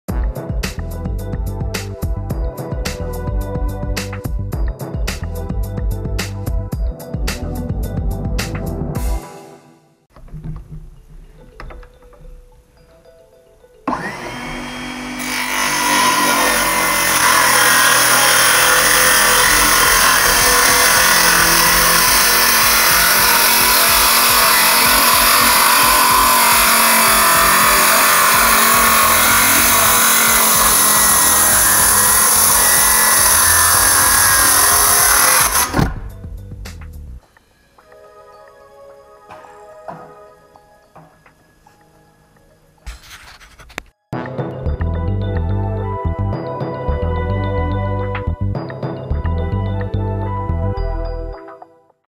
Using a useless tool
A demo of recent Useless tool , original  18 Volt  rated tools , with the after market adapter  to convert to the newer 20 Volt battery standard for Dewalt  Cordless tools.